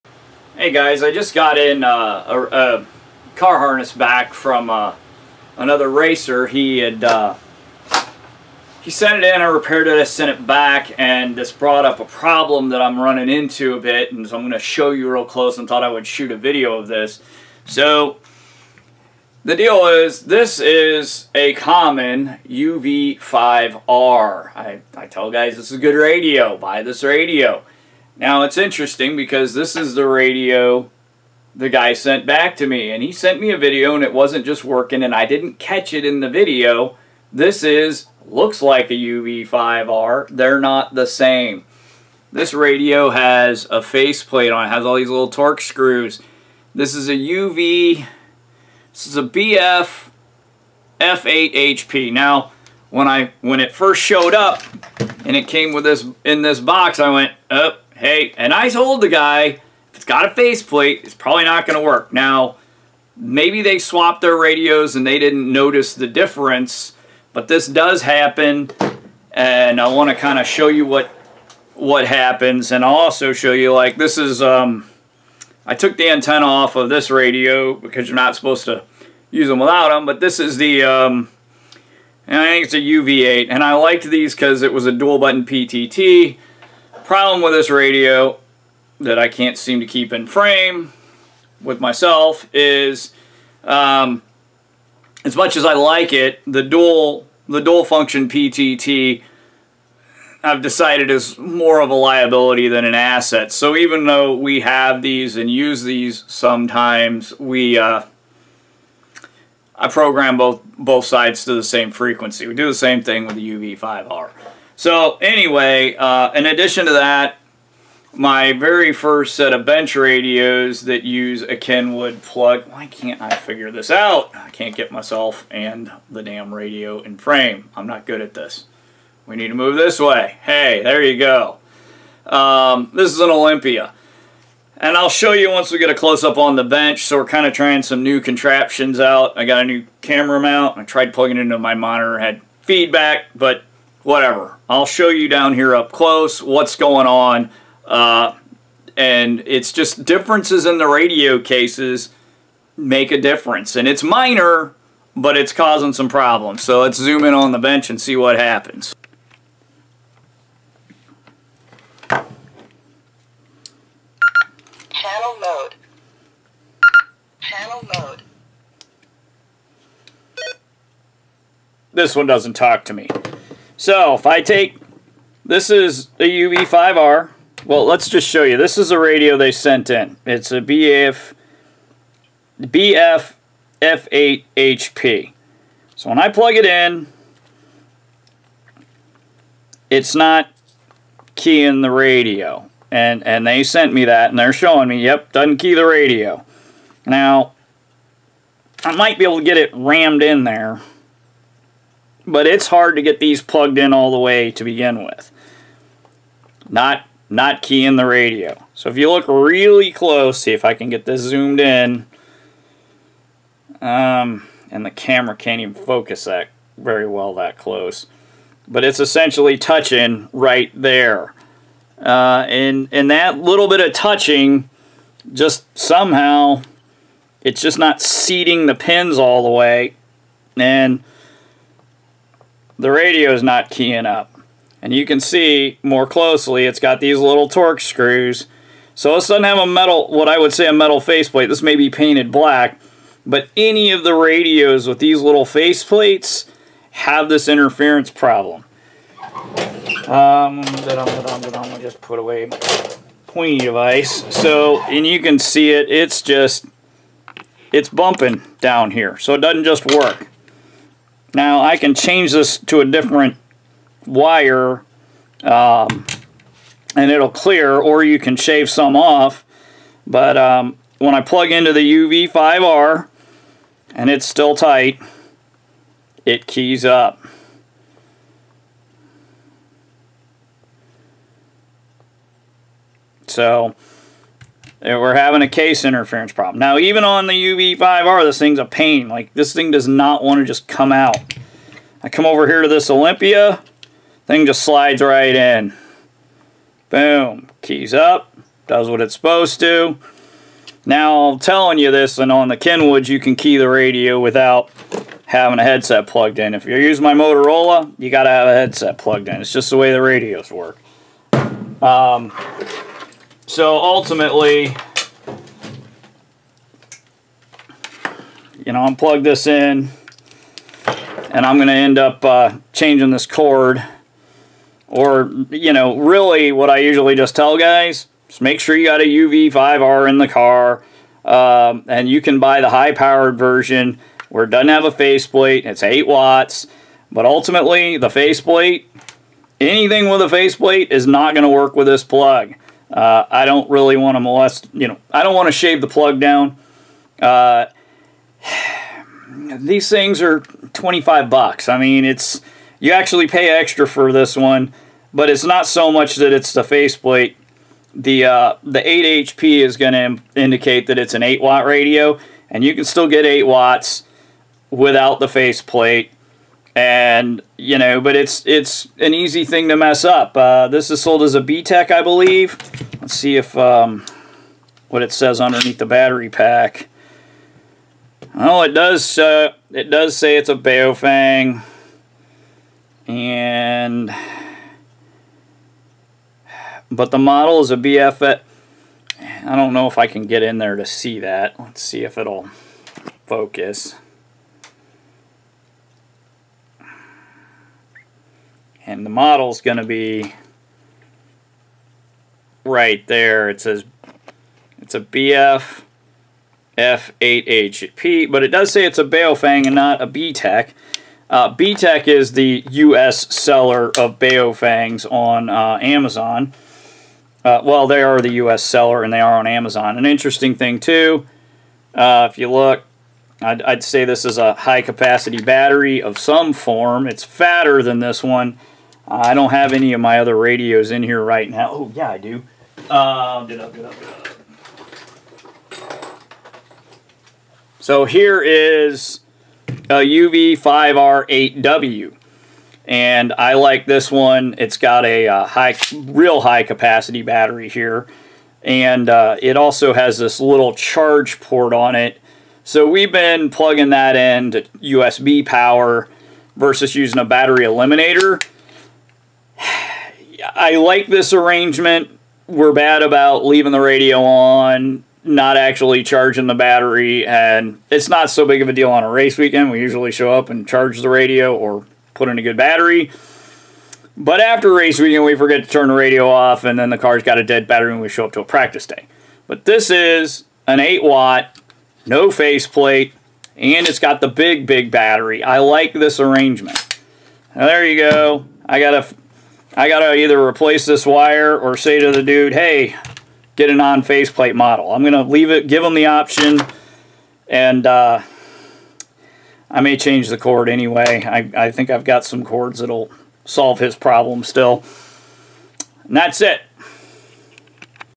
Baofengs UV-5R v UV-5R8W v BF-F8HP
Baofeng UV-5R style radios with faceplates have slightly different cases from radios without the faceplates which cause 2-pin accessory port problems.